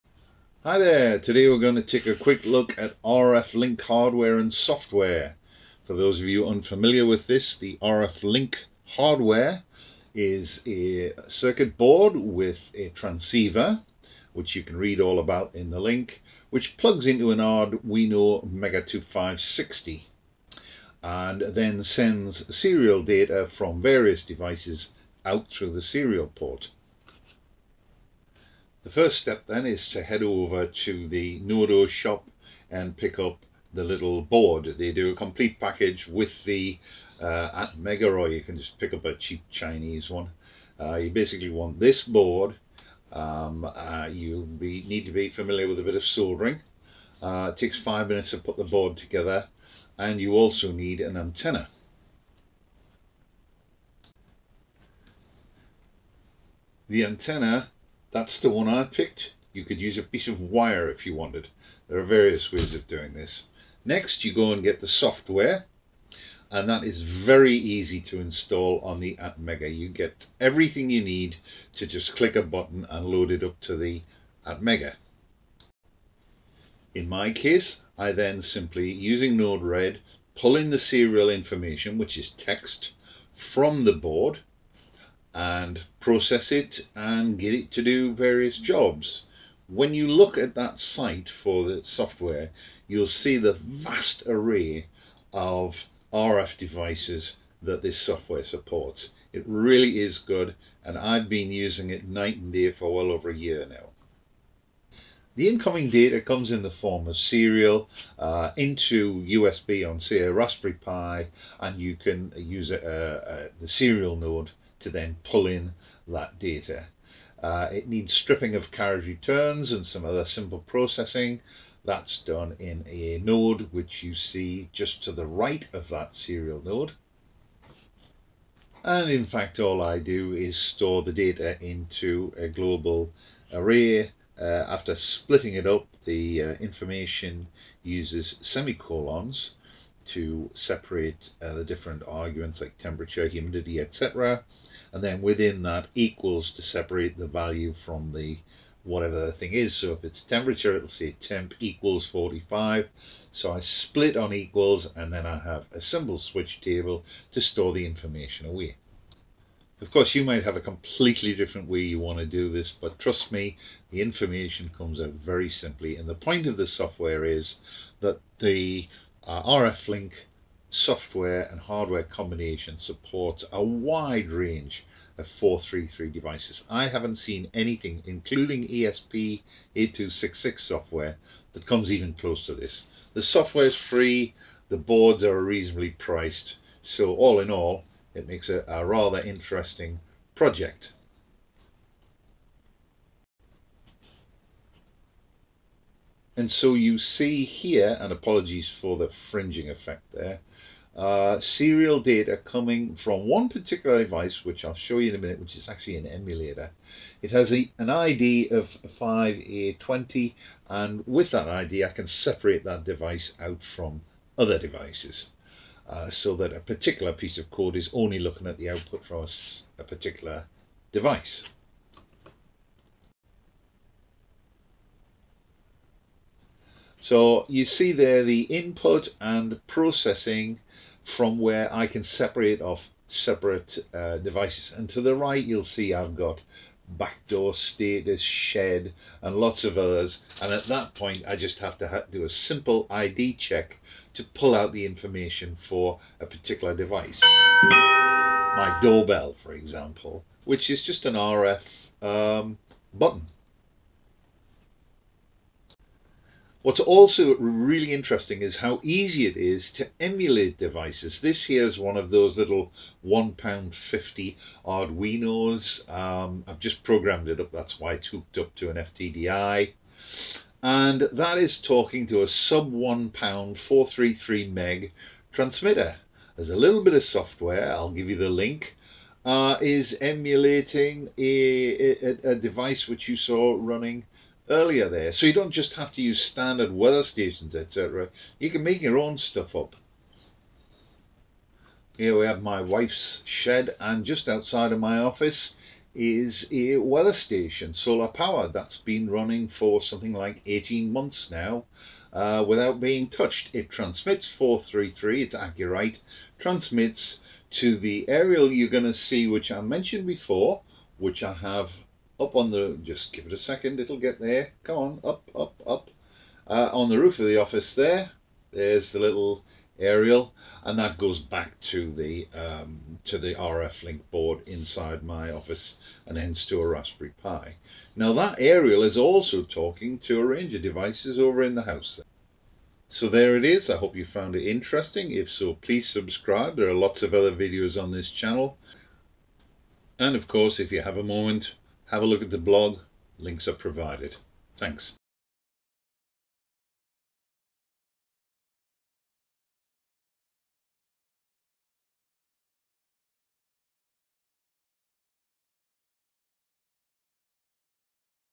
RFLINK in Action
RFLink describes the hardware and software making up an incredibly useful radio frequency receiver and transmitter setup for the hobbyist and for home control.

This video shows my setup with the RFlink board running on an Arduino Mega2560 board - and talking to Node Red. It also shows a demo Arduino board+transmitter emulating a standard weather station and the RFLINK board picking up and processing that information.